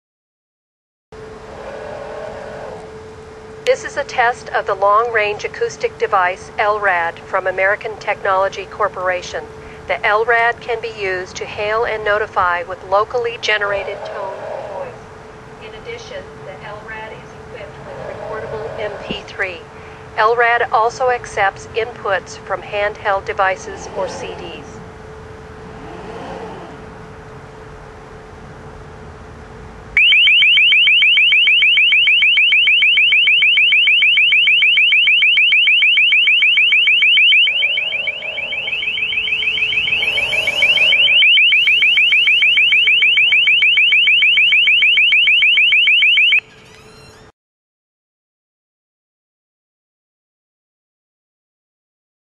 LRAD-RX Remote Long Range Acoustic Hailing Device
LRAD-RX remote operation demonstration. LRAD-RX can be operated remotely across an IP network enabling system operators to respond to potential threats from a safe environment while creating a complete unmanned perimeter security solution. LRAD-RX accepts intruder position information from other IP-enabled sensor systems to automatically find and track potential threats. LRAD-RX provides a large safety zone to give security personnel additional time and valuable information if an elevated intruder response becomes necessary.

LRAD-RX features an emitter head, integrated camera, high-intensity searchlight (optional) and LRAD Corporation's full pan and tilt capability for precise aiming and tracking. Audio output from LRAD-RX is highly directional so nearby personnel and system operators are not exposed to excessive audio levels, while still providing exceptional voice intelligibility and tonal clarity over long distances.